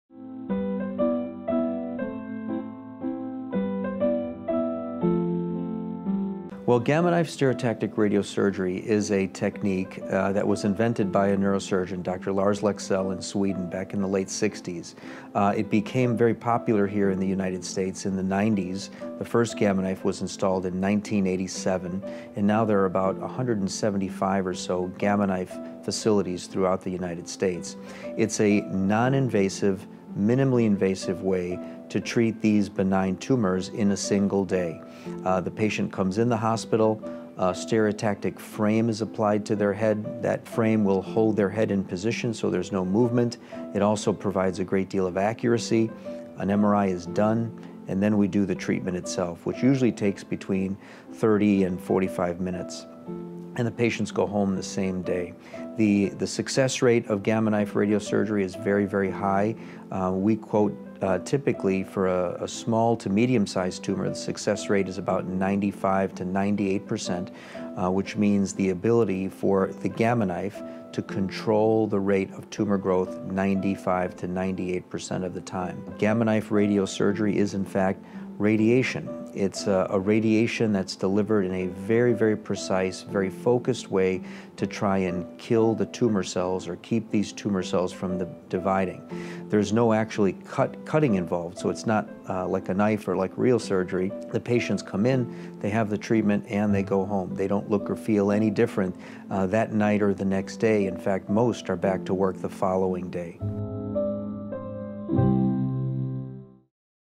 Gamma Knife Surgery, Explained — AMITA Health
Is Gamma Knife surgery right for you?

George Bovis, MD, an AMITA Health neurosurgeon based in Chicago’s northwestern suburbs, explains Gamma Knife stereotactic radiosurgery, from its invention by Lars Leksell in Sweden in the 1960s to its widespread adoption in the U.S. during the 1990s. There are currently about 175 Gamma Knife centers throughout the nation.

Why is the treatment so popular among neurosurgeons? “It’s a noninvasive/minimally invasive way to treat benign tumors in a single day,” says Dr. Bovis. A frame holds the patient’s head in position to ensure accuracy. After magnetic resonance imaging (MRI) is performed, the treatment itself takes between 30-45 minutes. Gamma Knife uses precisely focused radiation rather than incisions in order to destroy brain tumor cells or keep them from dividing. The patient gets to go home the same day they receive treatment, and can return to work or school the very next day.

Gamma Knife boasts an extremely high success rate, according to Dr. Bovis. “Typically, for a small-to-medium-size tumor, the success rate is typically about 95-98%.”